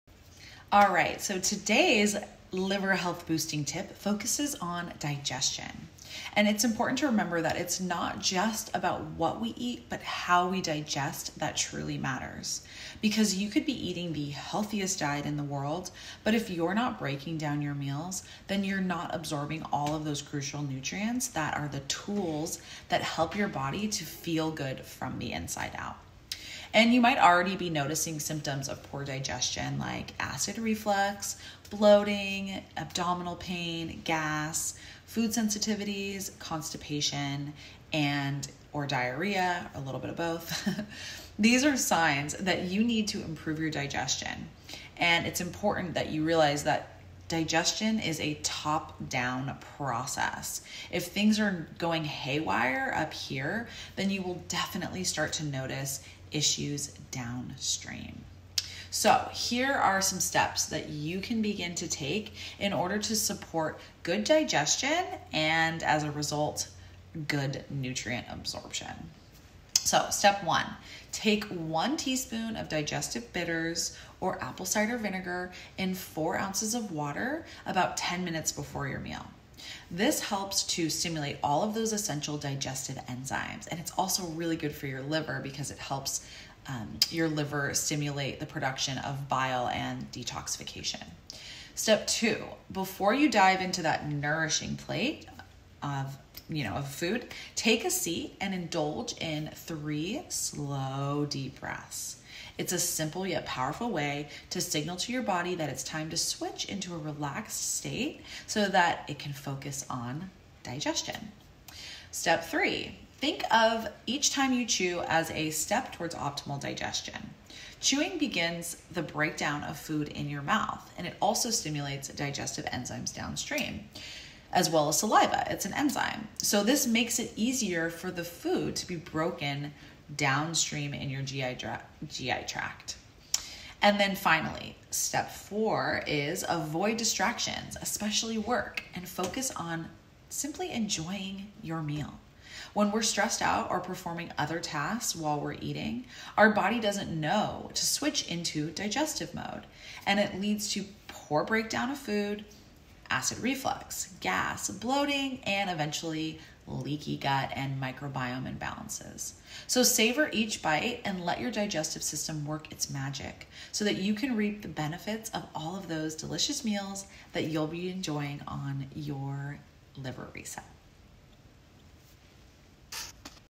Support Good Digestion
A well functioning digestive system is ESSENTIAL to ensuring that you are absorbing nutrients as well as to alleviating chronic digestive issues like acid reflux, bloating and IBS symptoms. Here are tips to help support good digestion. Here are 4 Steps that you can take to promote good digestion!